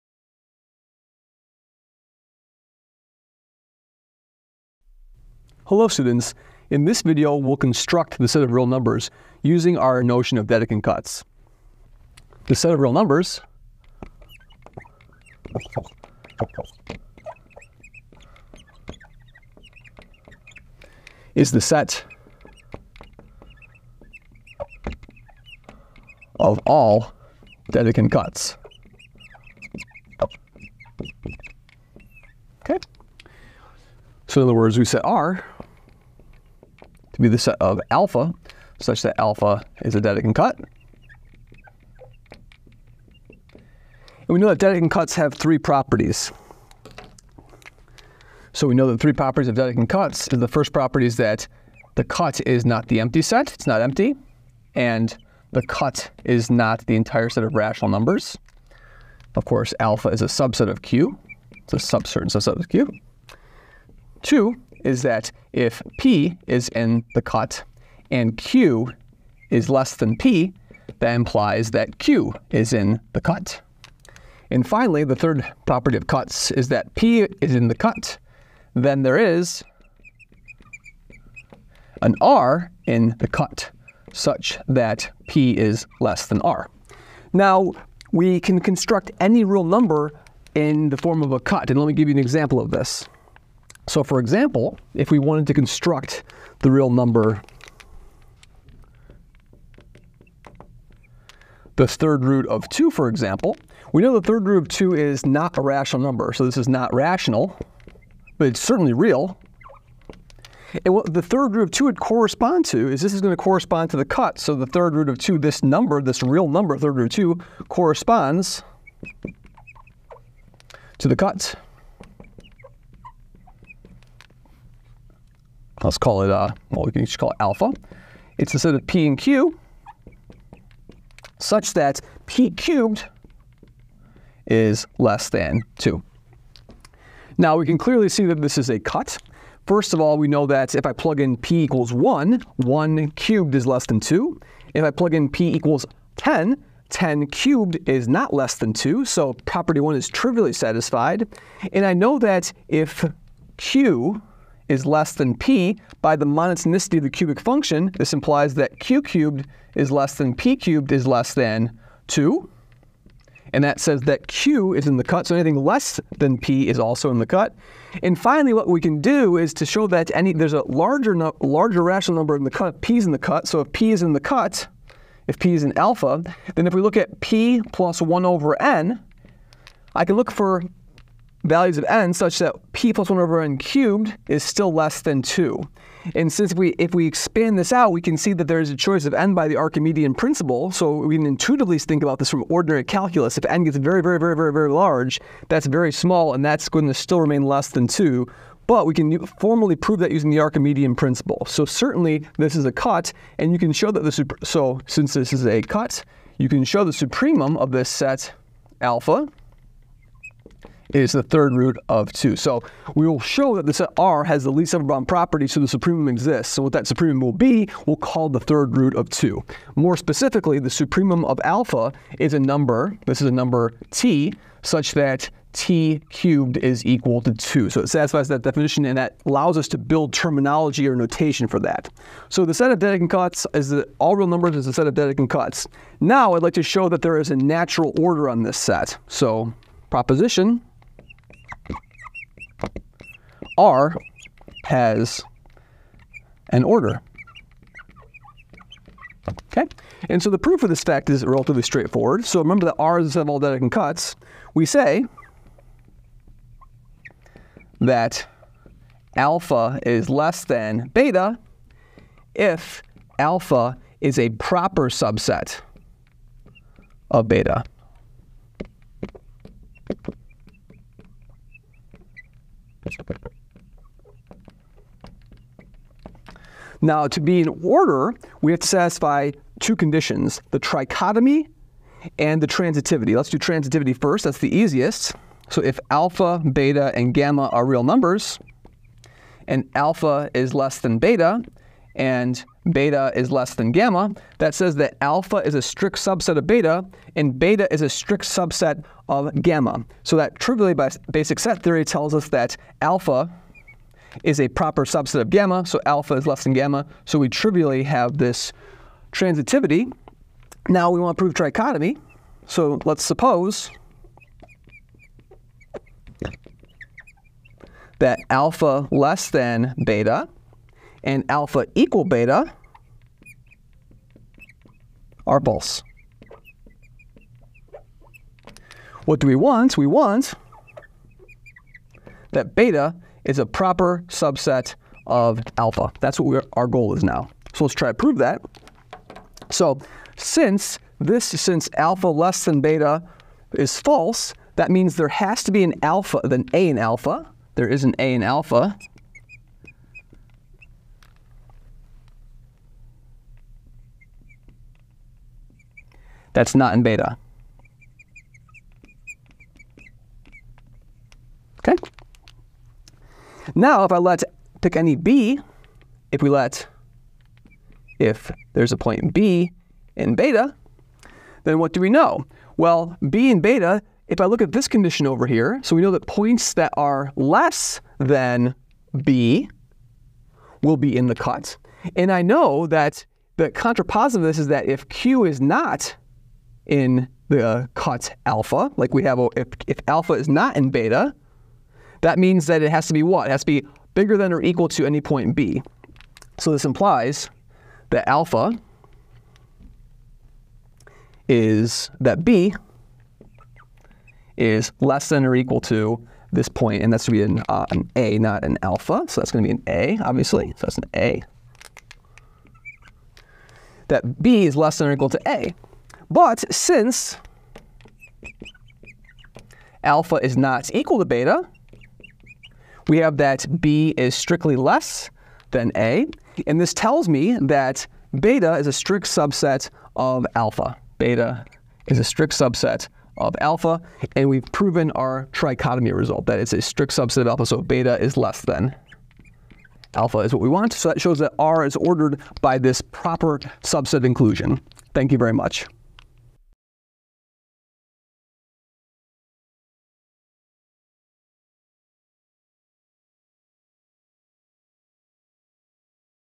Real Numbers as the set of Dedekind Cuts
We define the set of real numbers to be the collection of Dedekind cuts. We give an example of a Dedekind cut that corresponds to an irrational number.